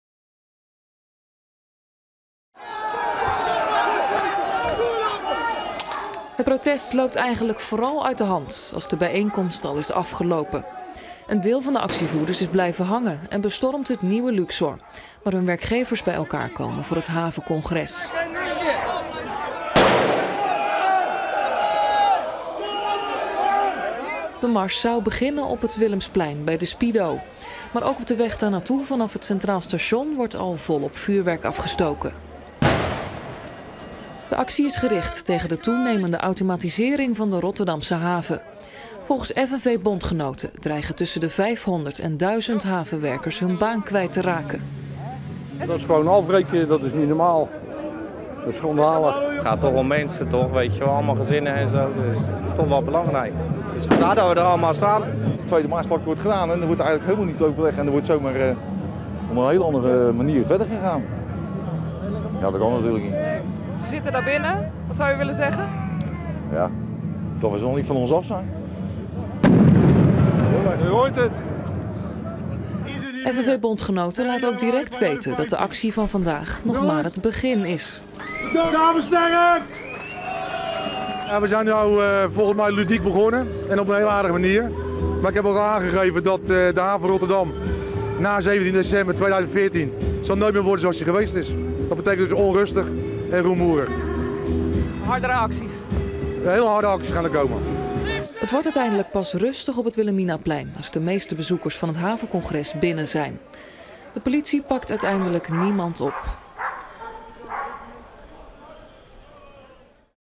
Grimmig protest havenwerkers in Rotterdam
Ongeveer duizend havenwerkers hebben woensdag gedemonstreerd tegen toekomstige gedwongen ontslagen. Het ging er af en toe grimmig aan toe. Bij het eindpunt van de mars op het Wilhelminaplein moest de ME kort ingrijpen. Er zijn geen aanhoudingen verricht.

   Lees meer: Grimmig protest havenwerkers in Rotterdam